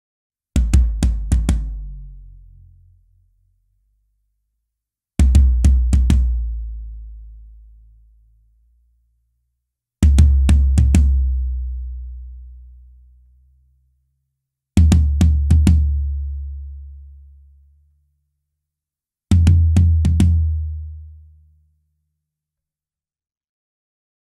EMAD + New 16” WTS Epiphany floor tom
If you’re looking for a thumpy, powerful floor tom tone that crosses into the world of modern metal/djent and even electronic bass drum sounds, the 16” UV EMAD from Evans Drumheads with our tuning system does the job! Providing the deepest, punchiest tone all the way up to resonant bass drops, WTS makes it easy to cover the full range of the EMAD in seconds with ease.

{WTS} 🎼🗝🥁